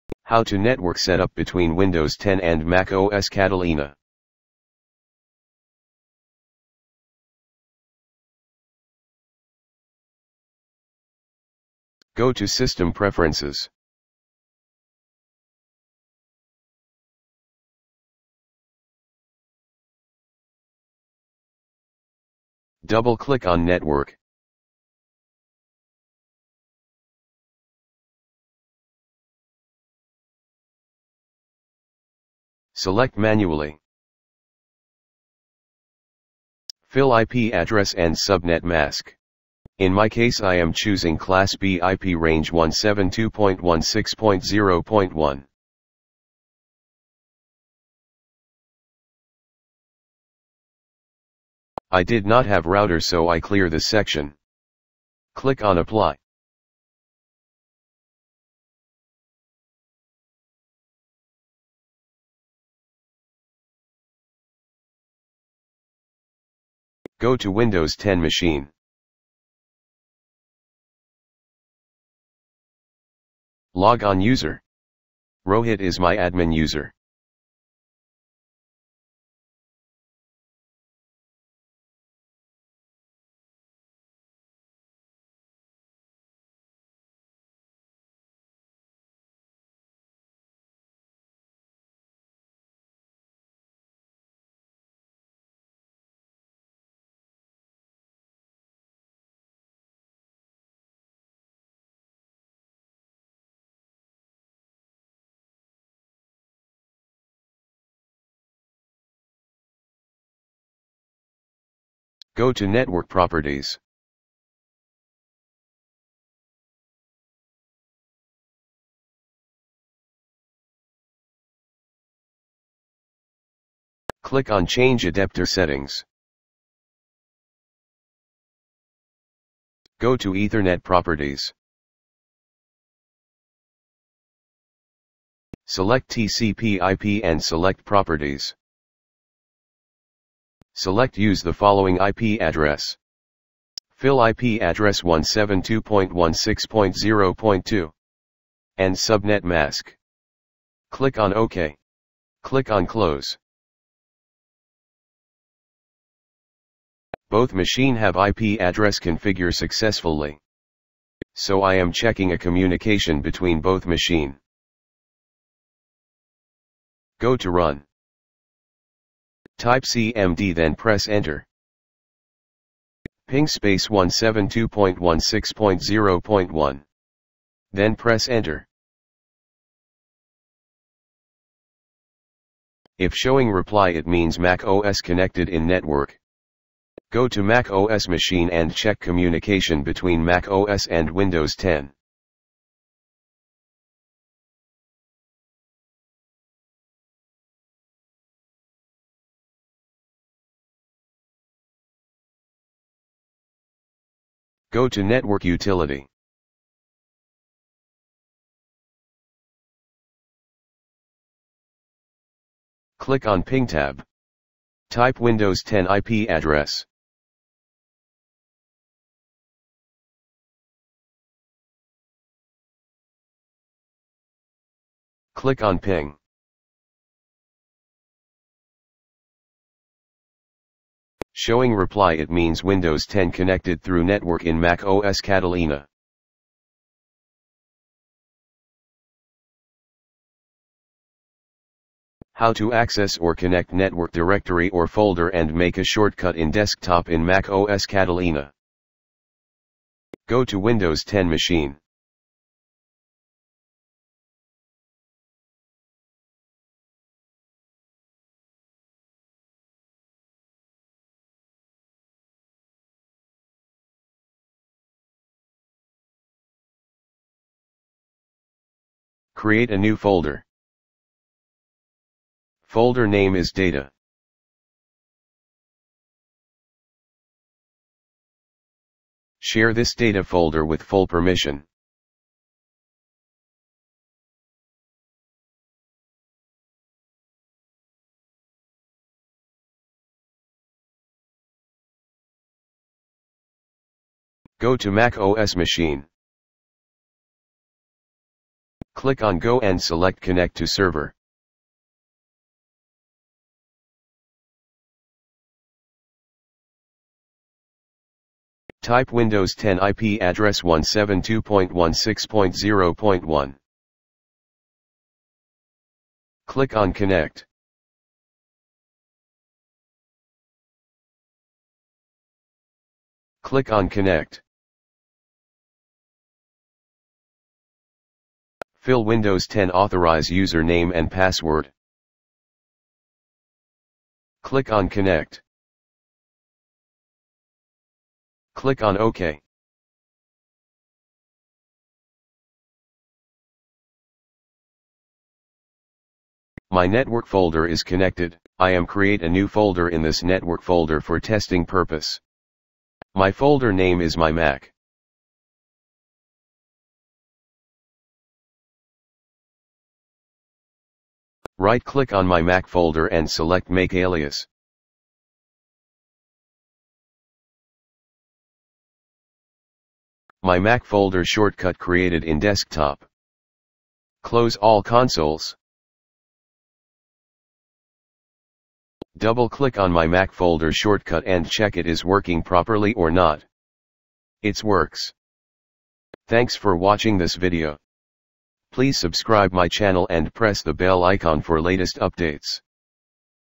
How To Network Setup Between Windows 10 & MacOS Catalina, Create Make Alias, IP Configure.
This Video is show on How To Network Setup Between Windows 10 & MacOS Catalina, Create Make Alias
+ IP Address Configure is macOs & Windows
+ Network configure between windows 10 & macOS catalina
+ Create network folder shortcut in mac
+ Connect Network in MacOS
+ Configure Apple Network